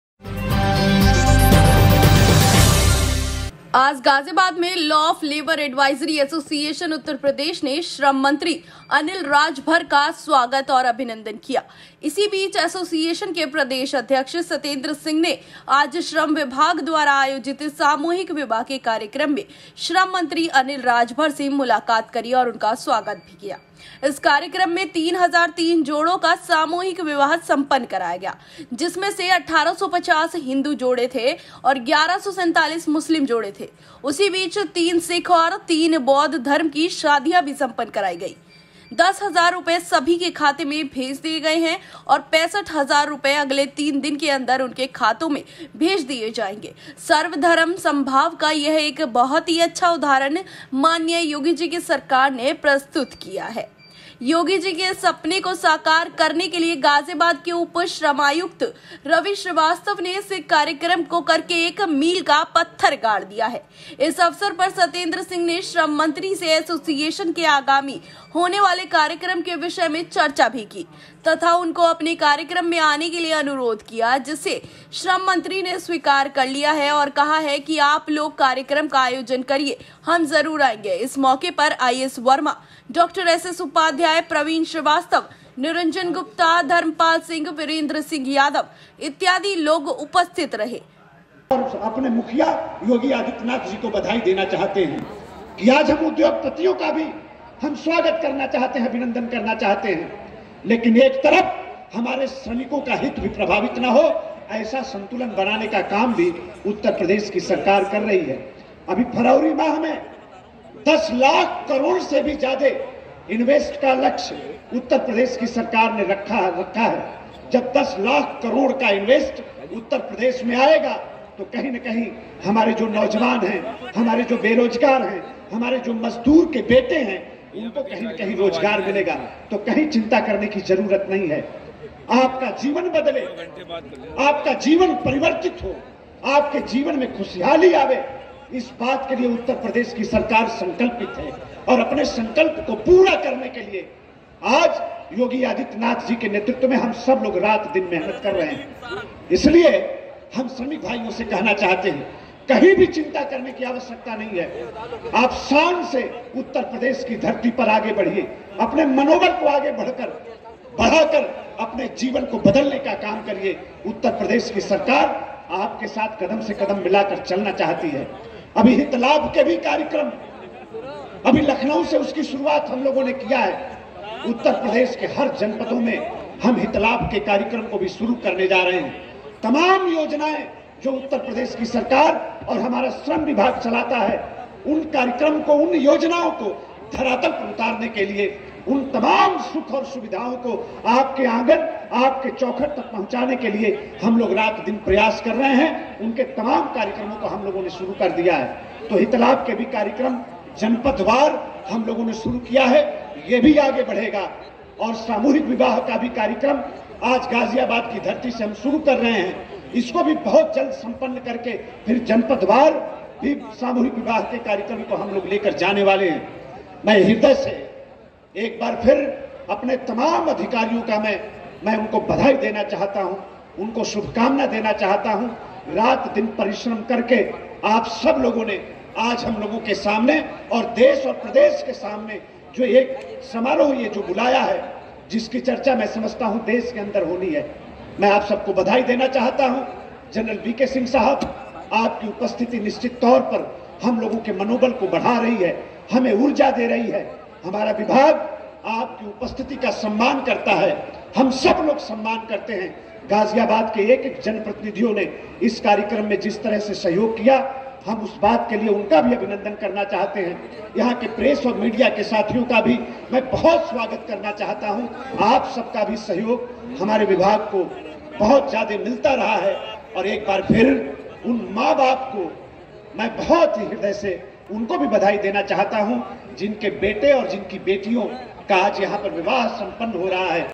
श्रम मंत्री का सत्येन्द्र सिंह ने किया स्वागत, कार्यक्रम में आने का दिया न्योता||UVINDIA NEWS
श्रम मंत्री का सत्येन्द्र सिंह ने किया स्वागत, कार्यक्रम में आने का दिया न्योता||UVINDIA NEWS||👉 Welcome to @UVINDIANEWS